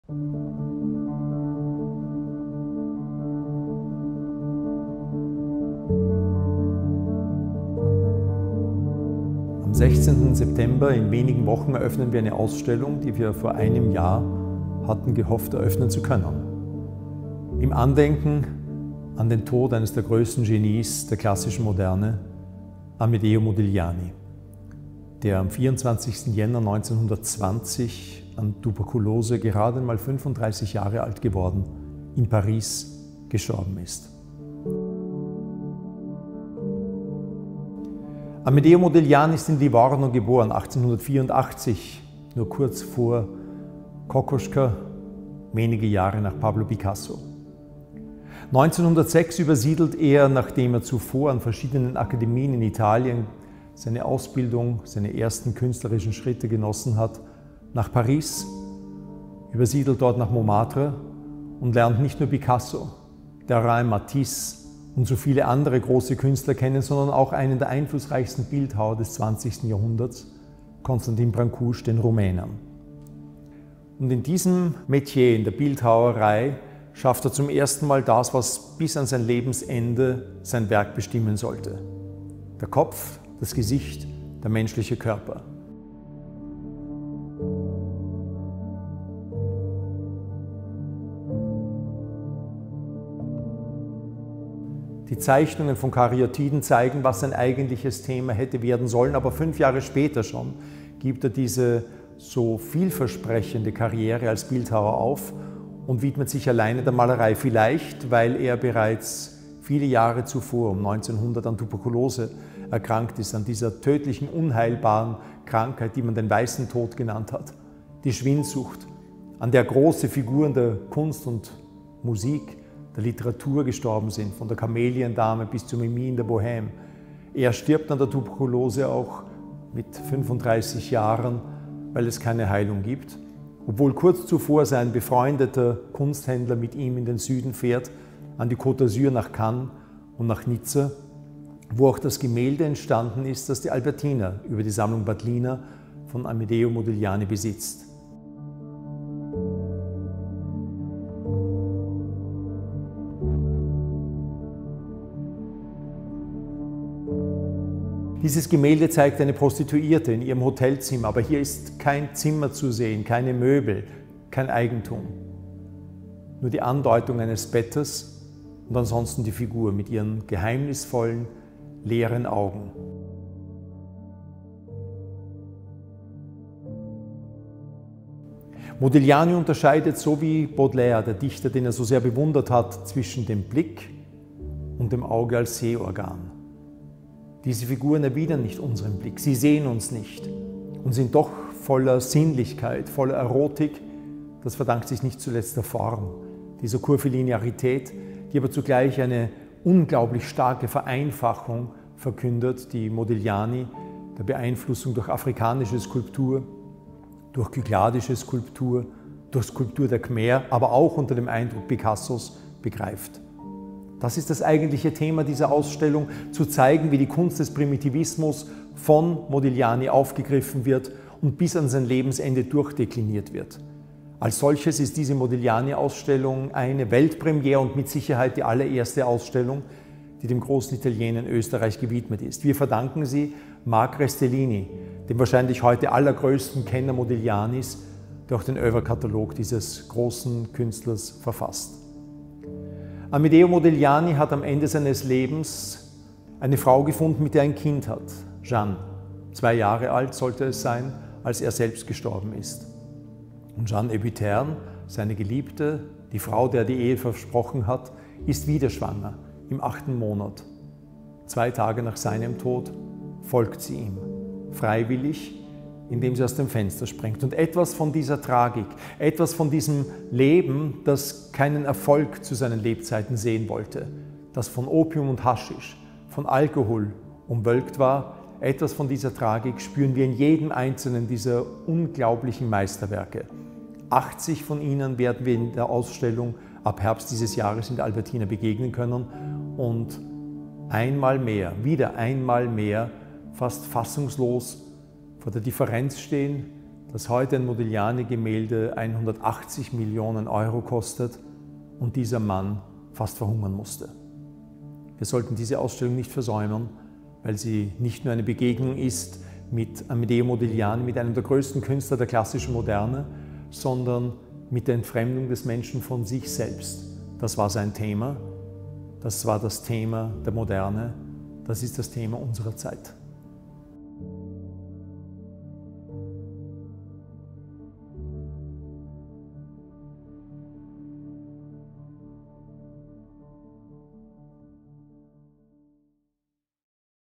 Modigliani at the ALBERTINA
Zum ersten Mal eröffnet diesen Herbst eine umfassende Ausstellung zu Amedeo Modigliani in Österreich. Und schon jetzt ist eines der Porträts des italienischen Malers in der Sammlung Batliner in der ALBERTINA zu sehen. Generaldirektor Klaus Albrecht Schröder bringt das einmalige Werk näher und erzählt, was Euch im September erwartet.
For the first time, a comprehensive exhibition on Amedeo Modigliani will open in Austria this fall. And one of the portraits of the Italian painter can already be seen in the Batliner Collection at the ALBERTINA. General Director Klaus Albrecht Schröder brings the unique work closer and tells you what to expect in September.